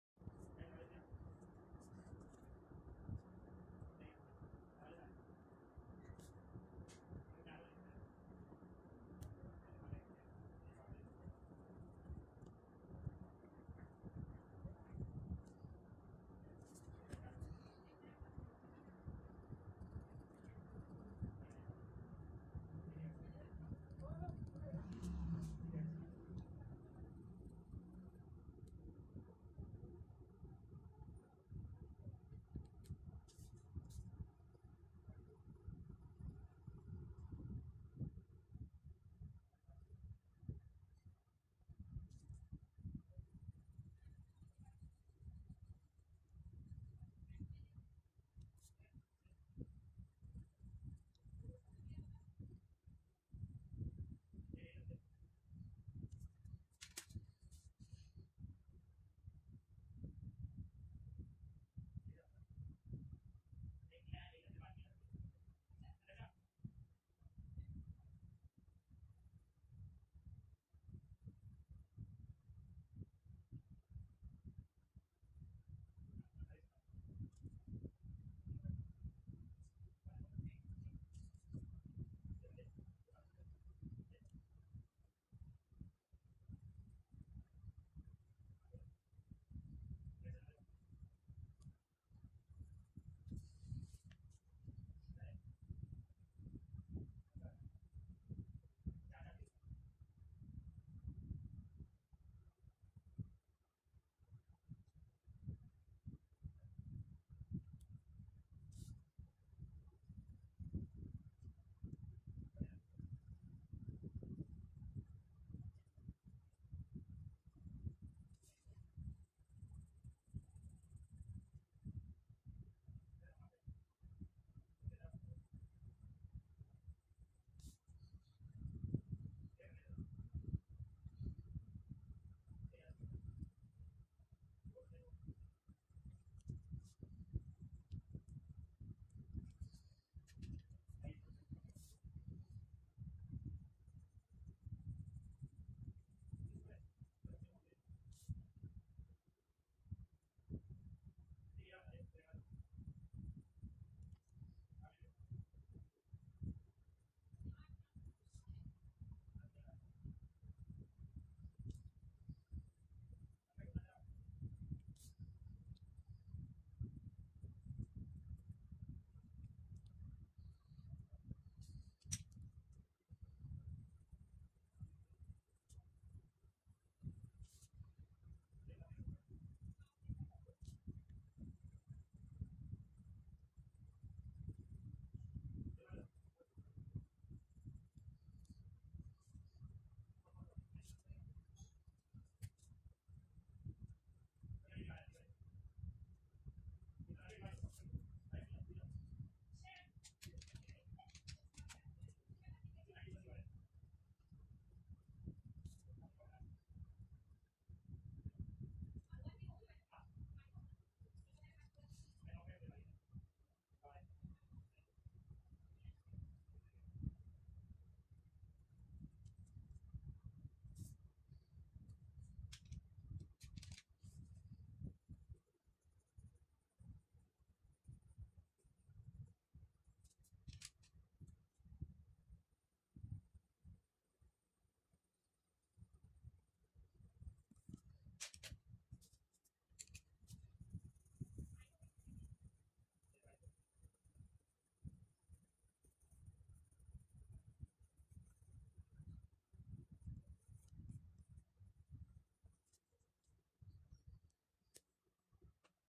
Secret Garden page - 1 (Coloring Sunflower)
Hello

I bought this book from Amazon, The quality of the paper is good but the color is quite yellowish.

Link for the book

The book is amazing for stress relief.

Should definitely try the coloring books from Johanna Basford, there are many more options and all her books are amazing.